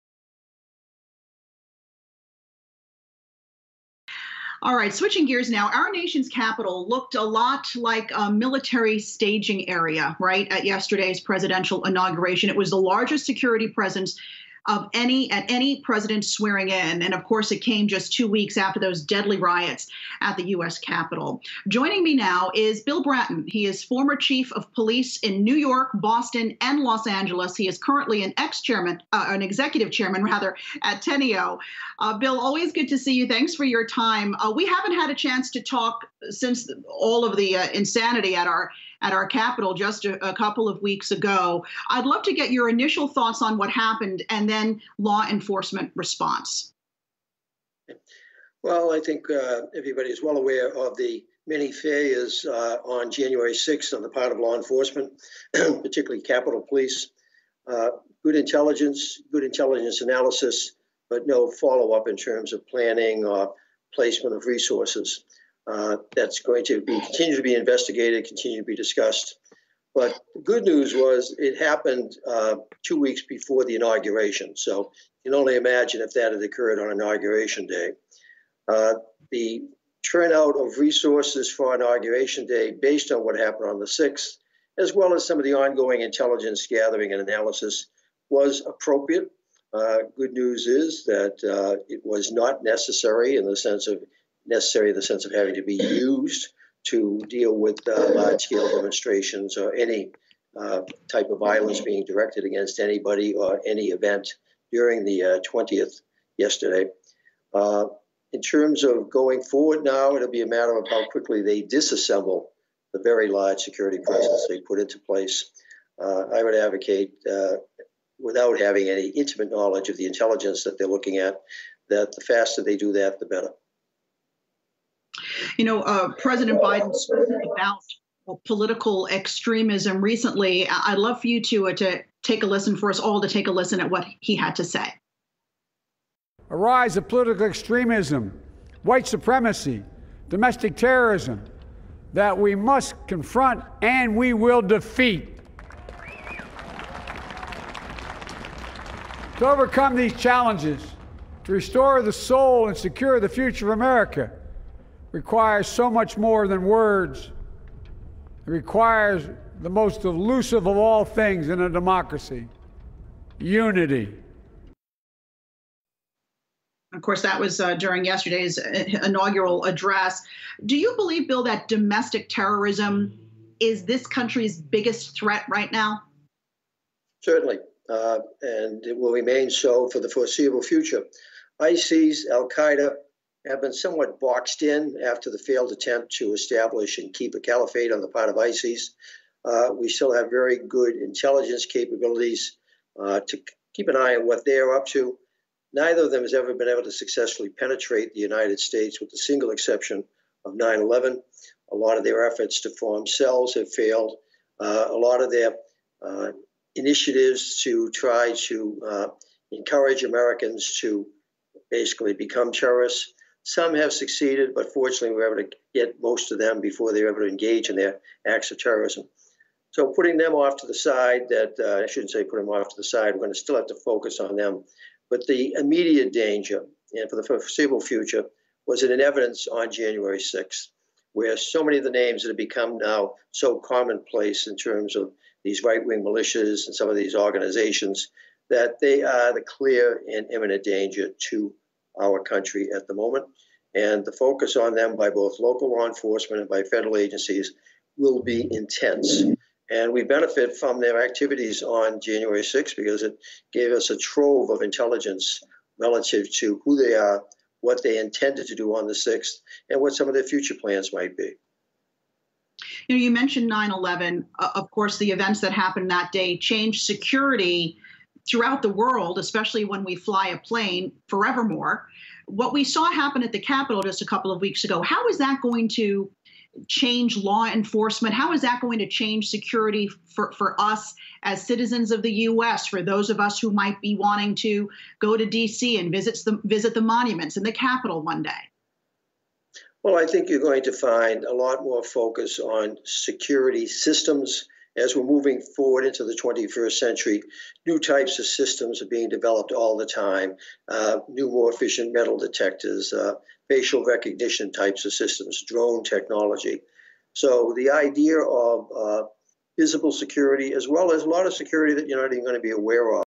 Former NYPD Police Commissioner Bill Bratton: Domestic terrorism is America's biggest threat
Yahoo Finance's Alexis Christoforous speaks with former NYPD Commissioner Bill Bratton about security threats in the United States.